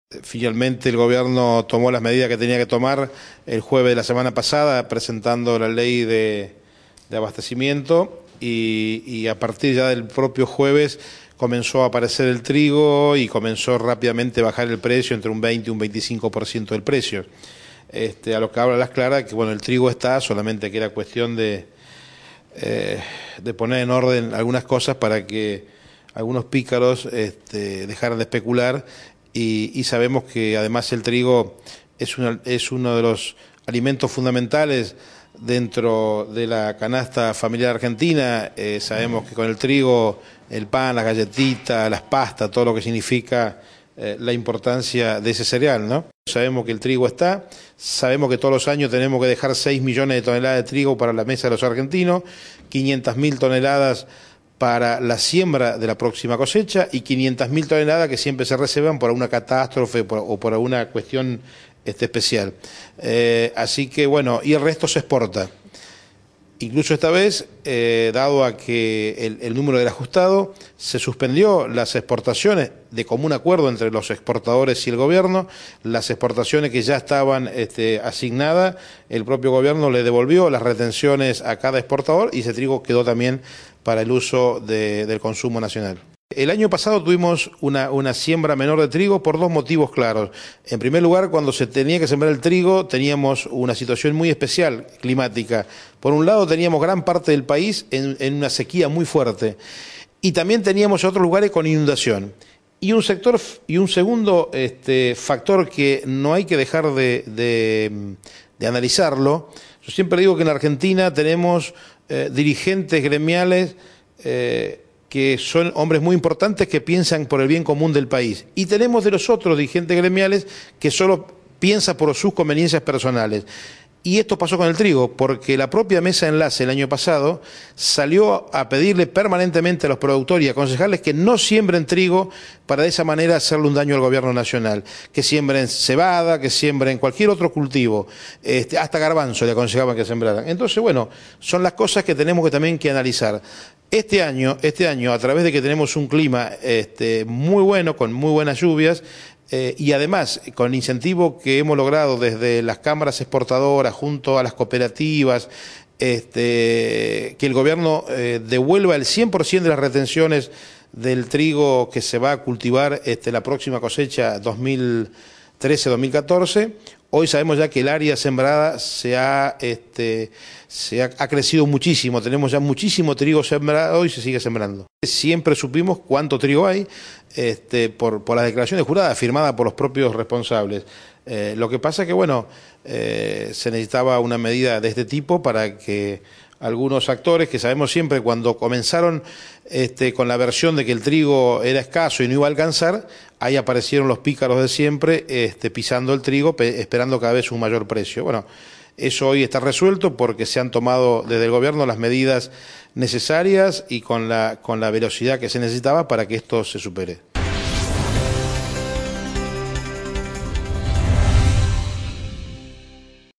En los últimos días el precio del trigo bajo 25% y aseguran abastecimiento normal...
...a la Industria Molinera
El sector molinero, los exportadores de cereales y la industria panadera ratificaron hoy que "está asegurado el abastecimiento" de harina a nivel local, al tiempo que subrayaron que en los últimos días bajó 25% el precio del trigo, como consecuencia de una mayor oferta del grano.
El nuevo cuadro debería determinar un retroceso en el precio que registró la harina durante el último tiempo, lo que permitirá además asegurar la provisión al público de un pan de cinco piezas a 10 pesos el kilo y que se retrotraigan los valores del resto de los productos de origen farináceo.
En una conferencia de prensa brindada este mediodía, Diego Cifarelli, titular de la Federación Argentina de la Industria Molinera (FAIM), atribuyó la regularización a una mayor oferta de trigo, tanto de parte de los productores como de los exportadores.
Según Cifarelli, "el abastecimiento era cuasi normal, hasta que en un momento de junio el cereal comenzó a superar los valores internacionales y empezaron a mermar los stocks".
Por su parte, Alberto Rodríguez, presidente del Centro de Exportadores de Cereales (CEC), explicó que las firmas del sector habían comprado "un volumen en exceso de lo que el total de la producción permitía exportar".
Ese volumen, cercano a los 2 millones de toneladas, empezó a volcarse al mercado interno desde que se definió el saldo exportable efectivo que el país tenía de trigo", agregó Rodríguez.
En tanto, Ider Peretti, presidente de la Confederación General Económica (CGE), ratificó que "el trigo está", como lo comprueban las declaraciones juradas de productores, acopiadores y exportadores, al final de la última campaña.
No obstante sostuvo, "siempre aparece algún pícaro en la cadena, en cualquiera de los rubros, que pisó el trigo para lograr lo que pasó: mayor precio".
El molinero Cifarelli señaló que las 150 plantas que operan en el país "tienen como trabajo prioritario atender el mercado interno", y eso "venía ocurriendo en forma casi simétrica al año anterior".
Precisó luego que el 80% del valor de la bolsa de harina obedece a la materia prima, el trigo, "que en las últimas 72 a 96 horas tuvo un mercado mucho más dinámico, con muchísima más oferta, lo cual provocó una baja de 20 a 25% en el precio".
Rodríguez, del CEC, insistió en que "ese volumen extra que la exportación compró, ya se volcó al mercado y lo mismo se hará con los volúmenes remanentes que queden en poder de los exportadores".
El empresario añadió que "entre hoy y el miércoles" se entregará al secretario de Comercio Interior, Guillermo Moreno, la información solicitada sobre los stocks al 31 de enero pasado y los embarques registrados desde entonces hasta el 4 de julio.
Peretti consideró que, en este contexto, "no tiene sentido hablar de importación de trigo", ya que "las declaraciones juradas muestran que hay existencias en el país".
"La decisión del Gobierno nacional (de advertir sobre la aplicación de la ley de Abastecimiento) fue la más acertada, porque las cosas se normalizaron de inmediato", agregó el titular de la CGE.
Peretti recordó que durante la siembra de trigo de la última campaña "tuvimos zonas con intensa sequía y otras inundadas", mientras "algunos dirigentes hicieron una campaña muy intensa y desaconsejaron el cultivo".
Evaluó, sin embargo, que "este año la situación climática es mucho mejor, y además, con el incentivo de la devolución de las retenciones, ya sabemos que el área sembrada con el cereal es muy importante".